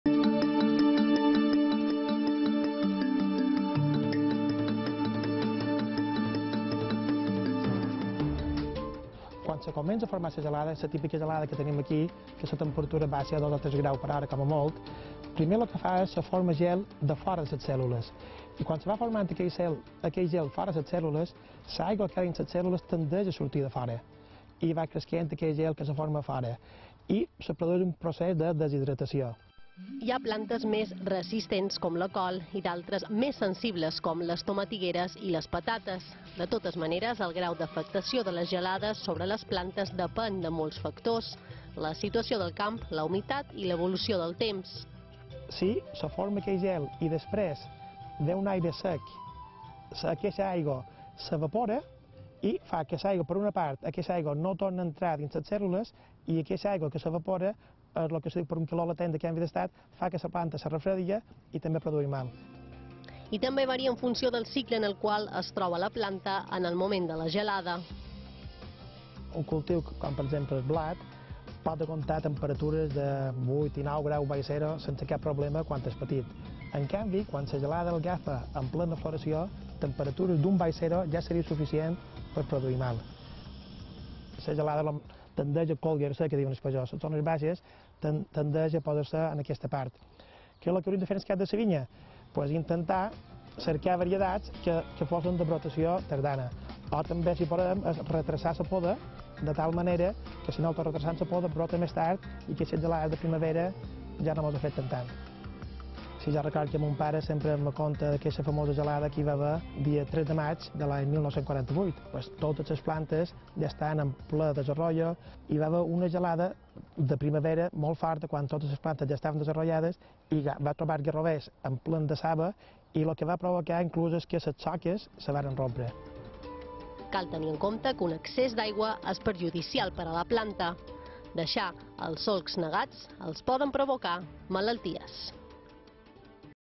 Les gelades afecten sobretot a les patates i les tomatigueres
Méteo, temps i natura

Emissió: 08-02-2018